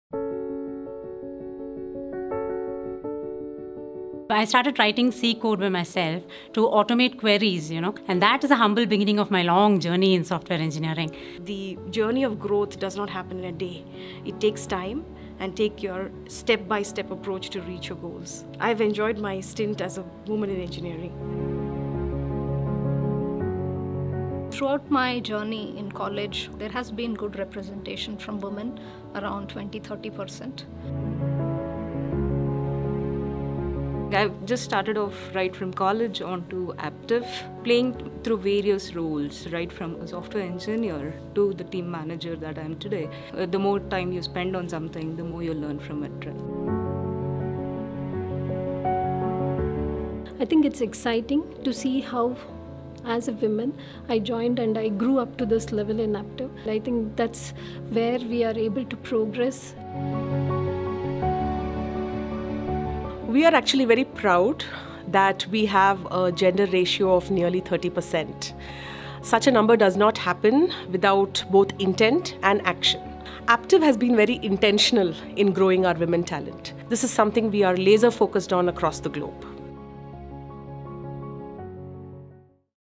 Women in Engineering
Aptiv India promotes gender diversity in the workplace and fosters an inclusive environment to ensure that our people have the support and opportunities they need to advance their careers and make a significant impact. Hear from some of the many talented female innovators leading the way here at Aptiv, and take the next step in your career with confidence!